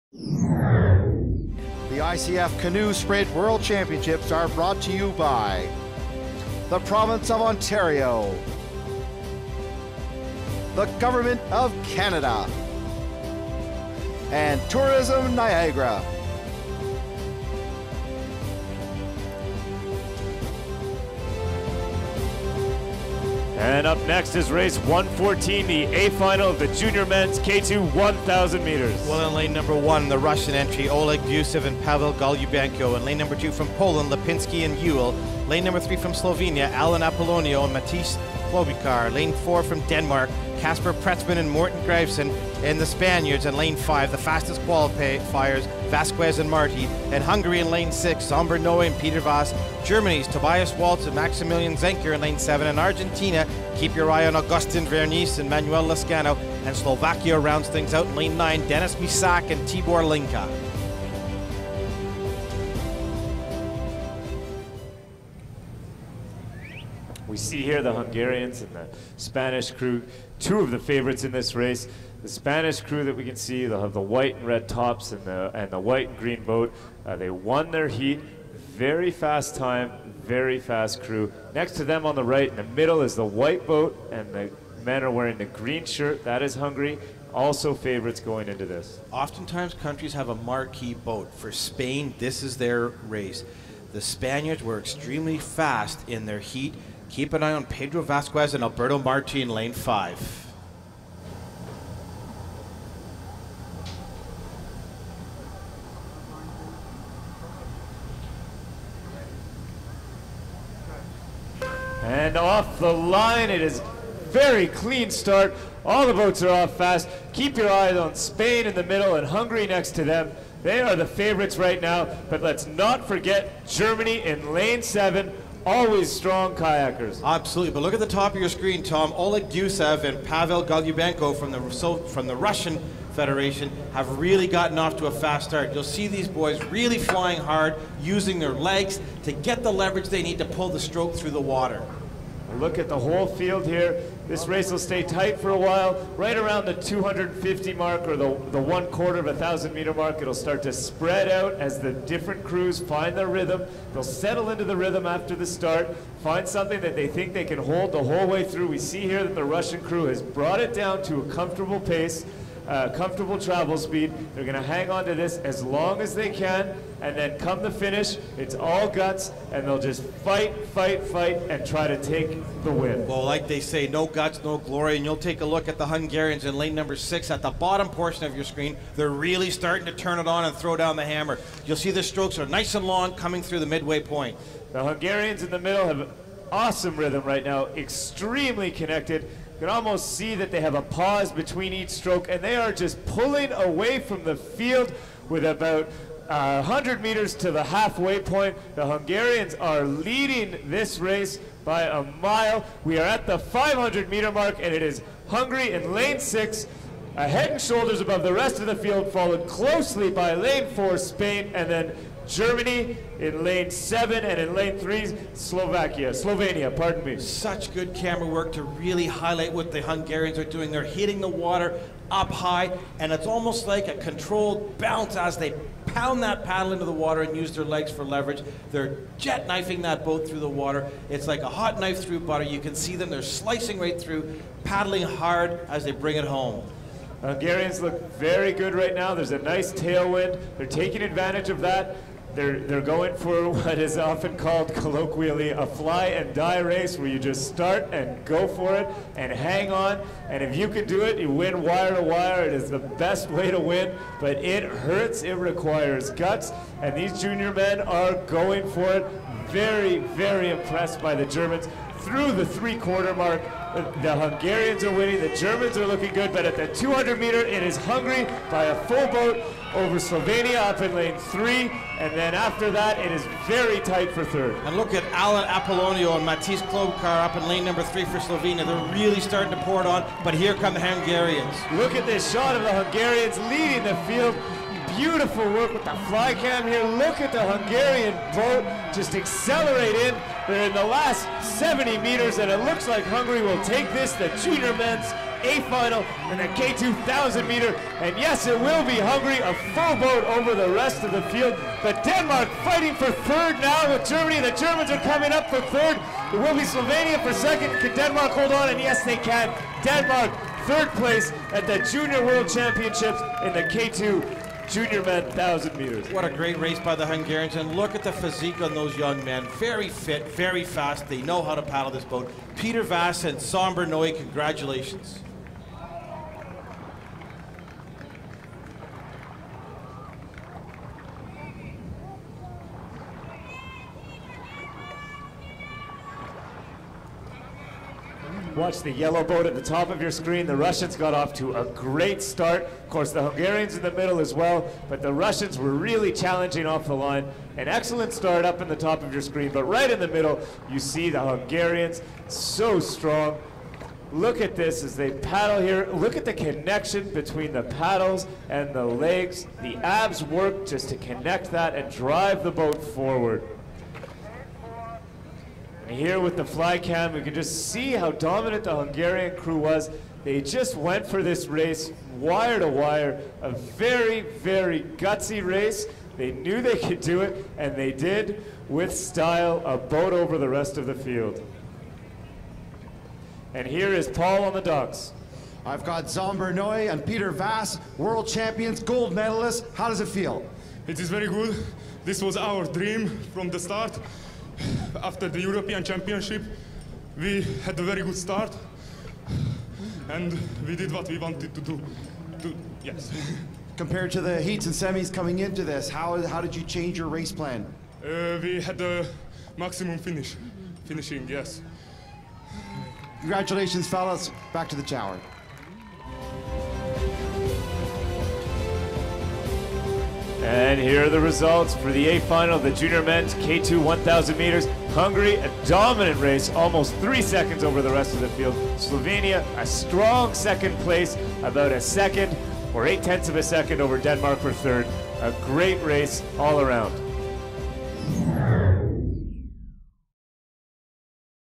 2013 ICF Junior & U23 World Championships Final A, K2 Jun Men 1000m Canoe Sprint
2013 ICF Junior and Under 23 Canoe Sprint World Championships
K2 Jun Men 1000m Final A
1º Zsombor NOE
   PÈter VASS          03:13.650 HUN
2º Alan APOLLONIO
   Matic KLOBUCAR      03:16.467 SLO
3º Casper PRETZMANN
   Morten GRAVESEN     03:17.651 DEN
4º Tobias WALZ
   Maximilian ZENKER   03:18.667 GER
5º Pedro V£ZQUEZ
   Albert MARTÕ        03:20.149 ESP
6º Dawid LIPINSKI
   Damian UHLE         03:20.568 POL
7º Denis MYSAK
   Tibor LINKA         03:21.004 SVK
8º Oleg GUSEV
   Pavel GOLUBENKO     03:21.487 RUS
00:07.837 RUS
9º AgustÌn VERNICE
   Manuel LASCANO      03:22.311 ARG